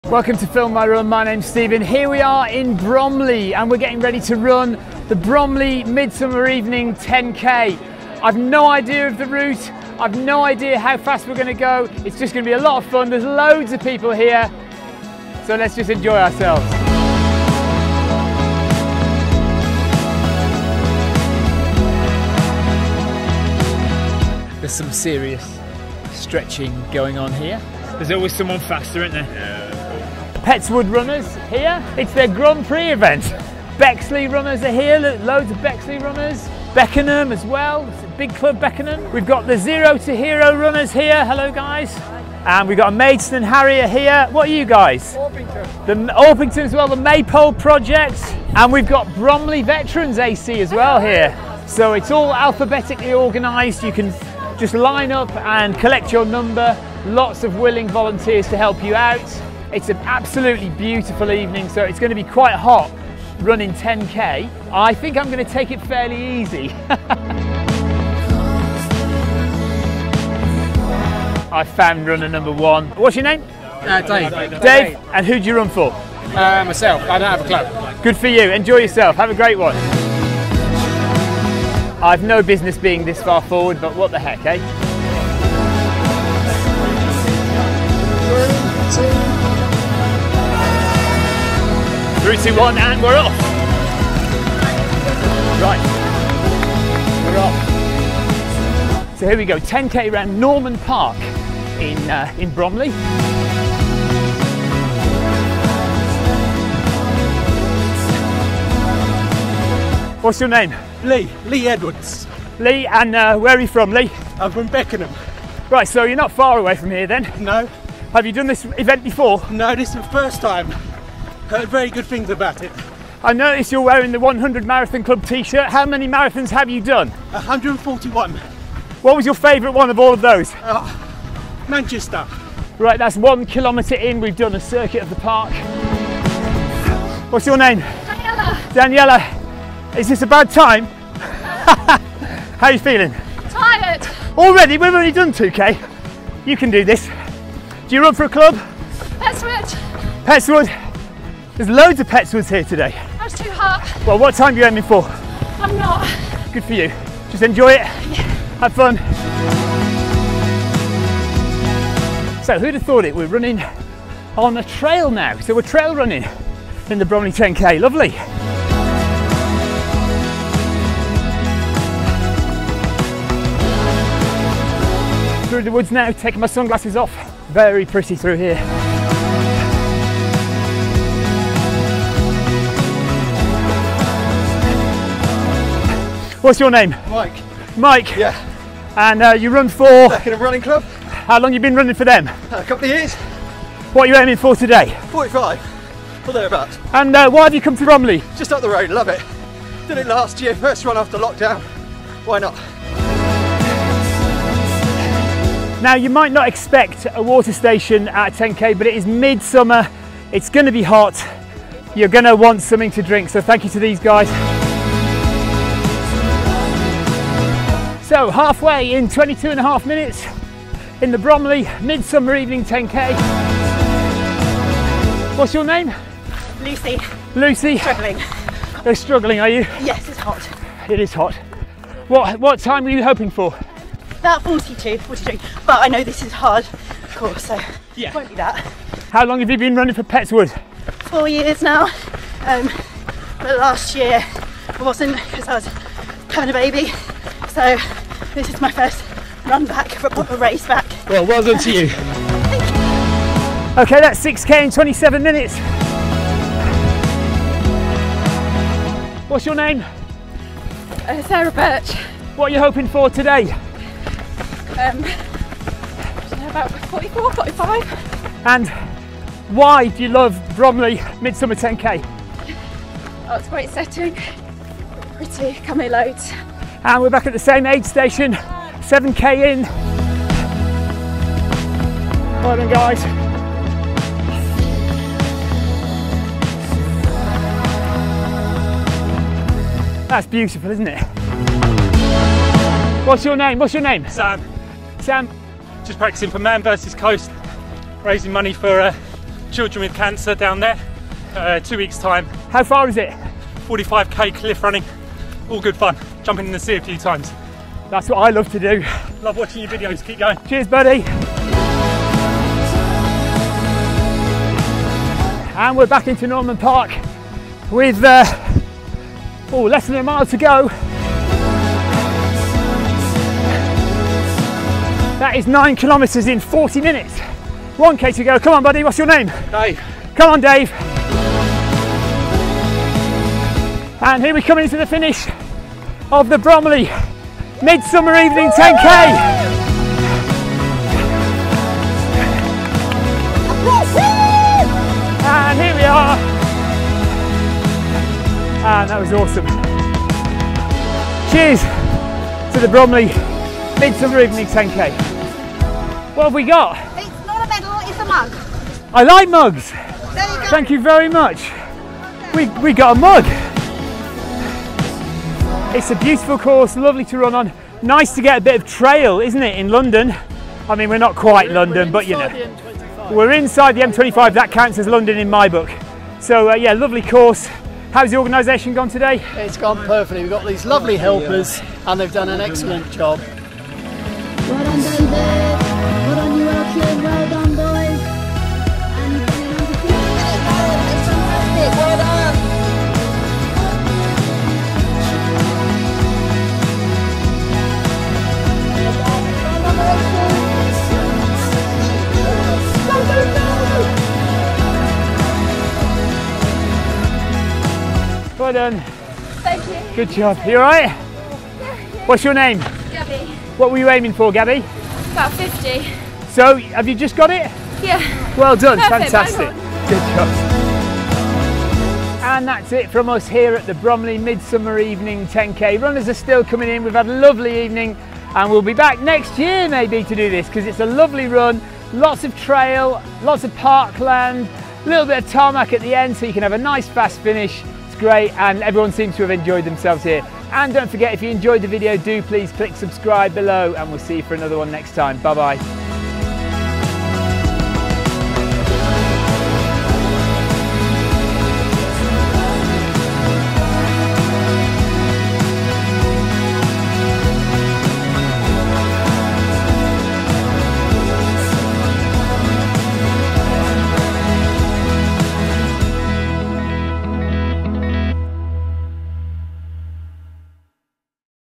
Bromley Midsummer Evening 10k | Film My Run
We were invited to Bromley in South London to run the Bromley Midsummer Evening 10k. It's a very attractive run through Norman Park and the surrounding countryside. I enjoyed it. I hope you do too.

The race was very well attended by numerous local running clubs. In particular Petts Wood Runners, Beckenham Running Club, Blackheath and Bromley Runners, Zero to Hero, Crystal Palace Ac and Maidstone Runners.